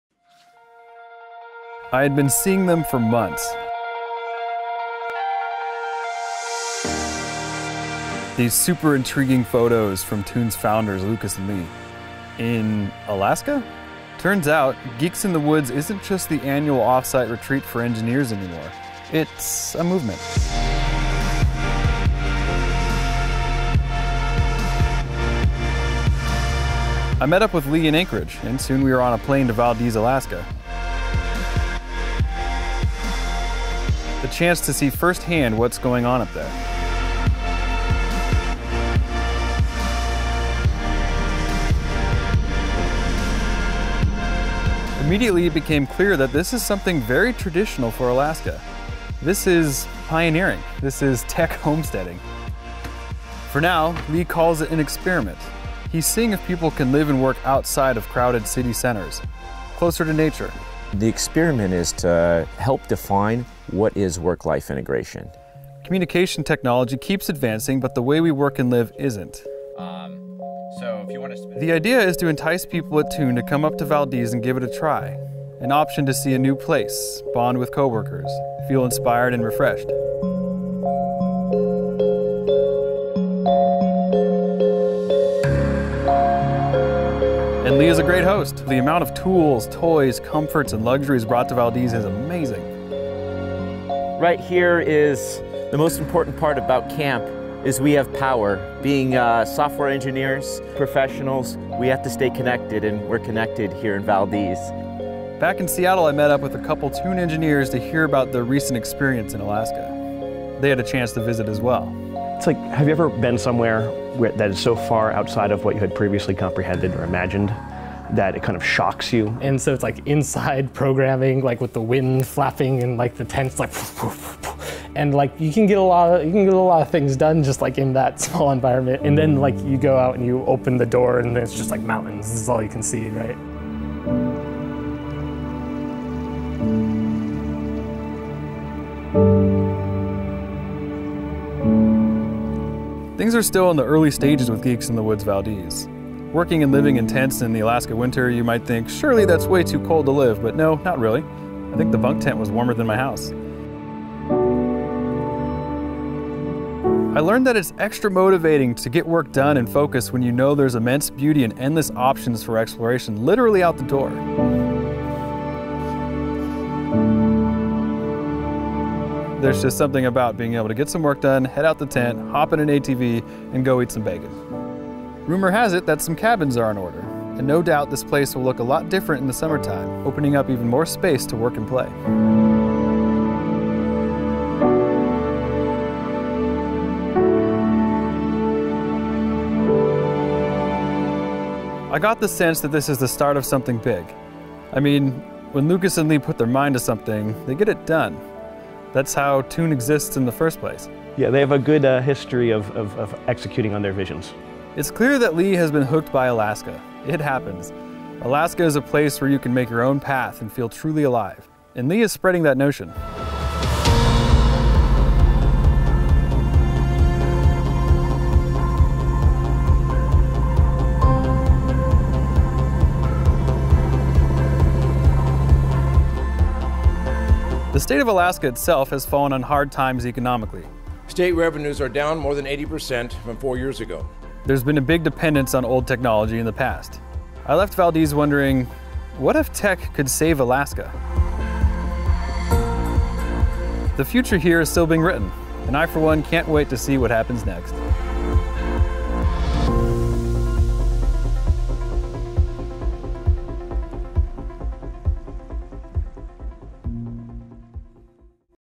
Tech in Alaska
A work life experiment pioneering Alaska’s next frontier - the tech frontier.